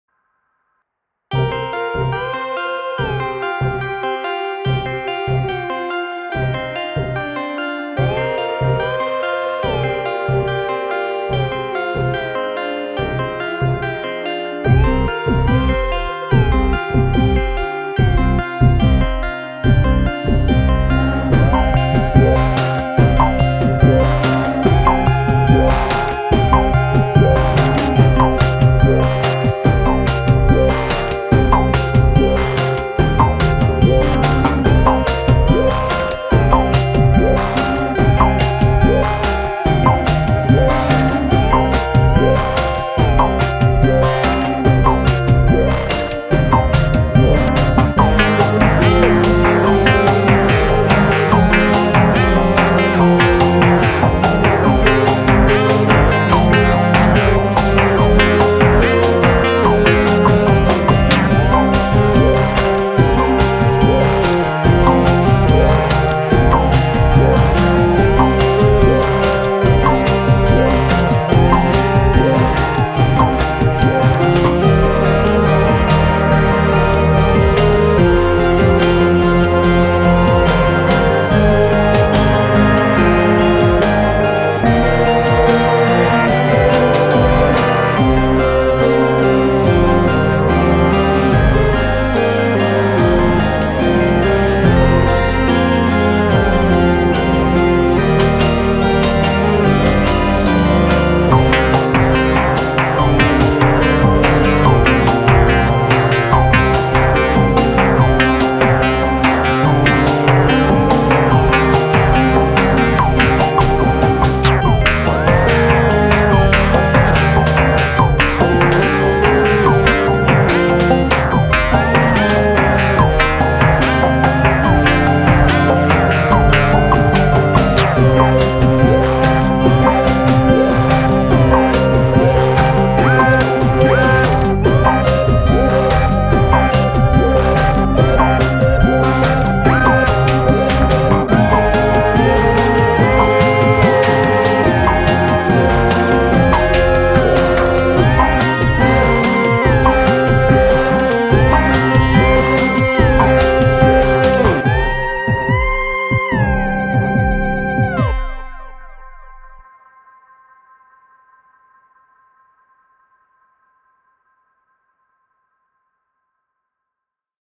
Playing a song with the Bass Station 2 with new sounds
Thanks to a new firmware update, the Bass Station 2 has got a whole lot more features than before.

I've worked on a bank of 100 sounds, to share around, but I may need to wait for a "Components" update to be able to save and share the AFX mode presets.
Here is the link to the 100 sounds (without AFX) in SysEx format.
Save your own presets first.

This is just a short improvised jam using a few of the sounds, some reverb and delay and some slight equalization and limiting.